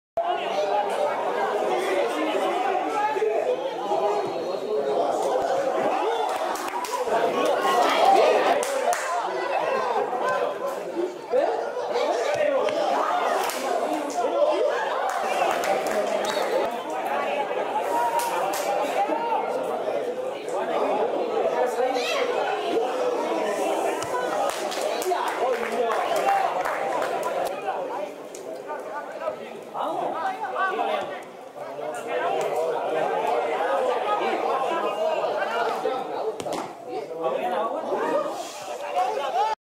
4 jugadas ofensivas del Arucas en el último partido contra el CD Vera. Tercera División Canaria
Partido de fútbol Arucas CF-CD Vera. Tercera División Canaria. 17/9/2016. Jornada 5. Temporada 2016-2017. Campo de Fútbol Municipal Tonono I. Arucas. Resultado final: 1-1.